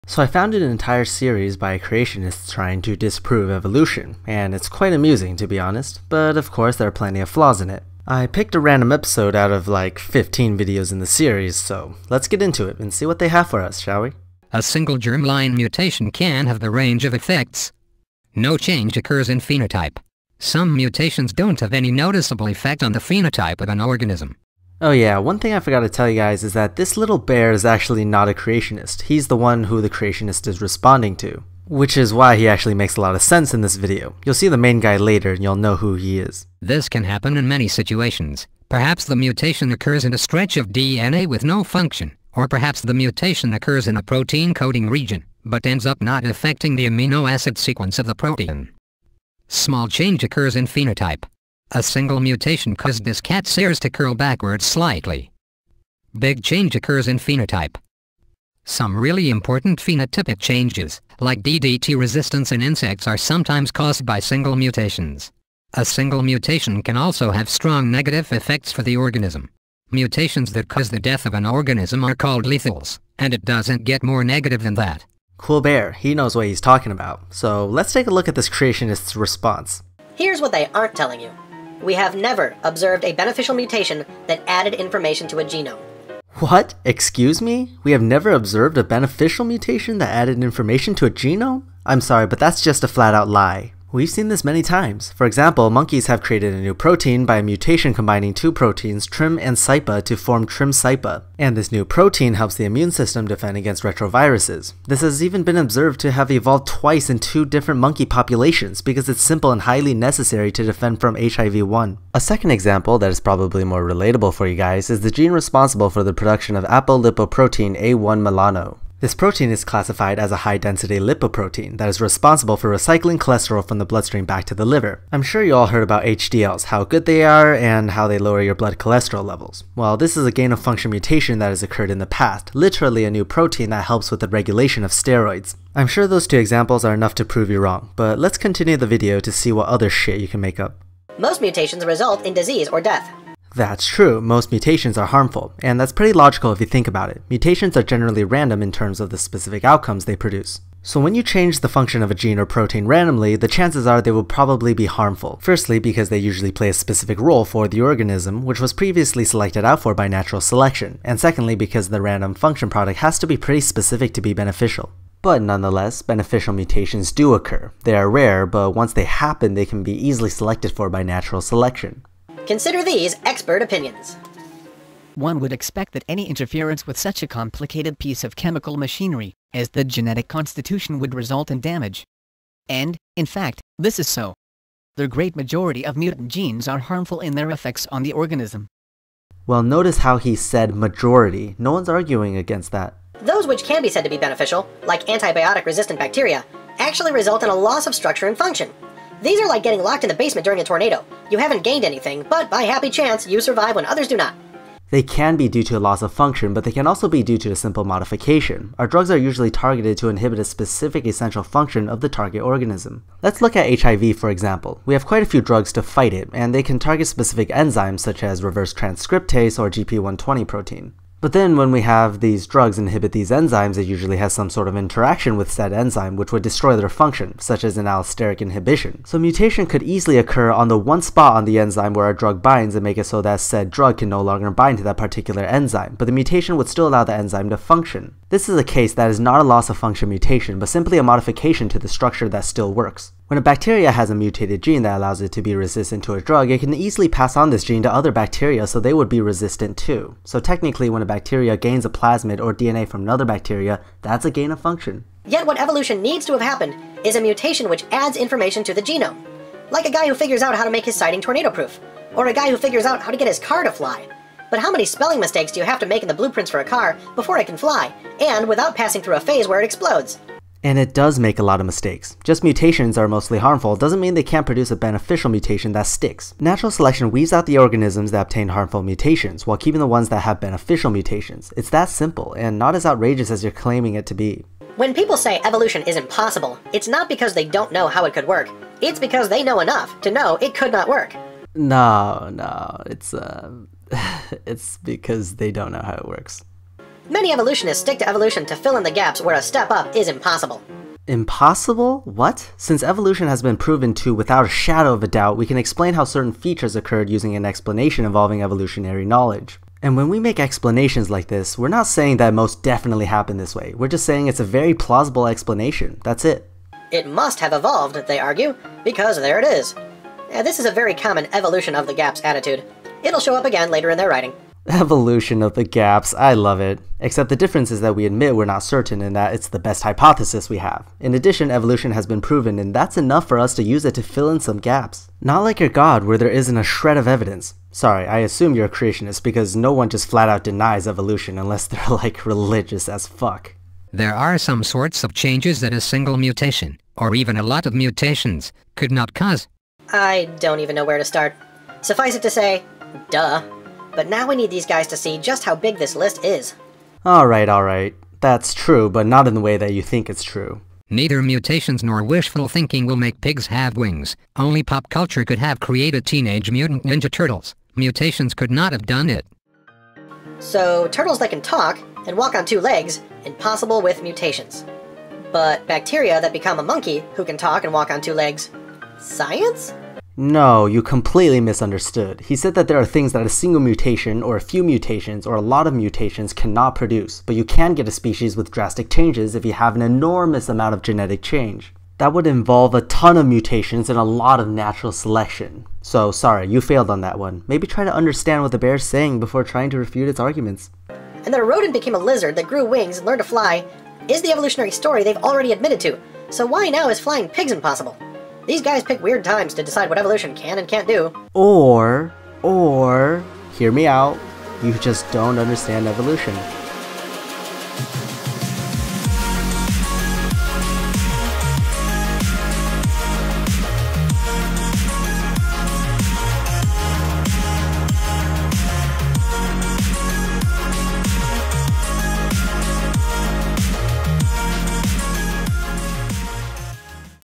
Creationist Needs a Gain of Function Mutation in His Brain
Do beneficial gain of function mutations occur? Can evolution be true since most mutations are detrimental to the organism? Find out today with Professor Stick!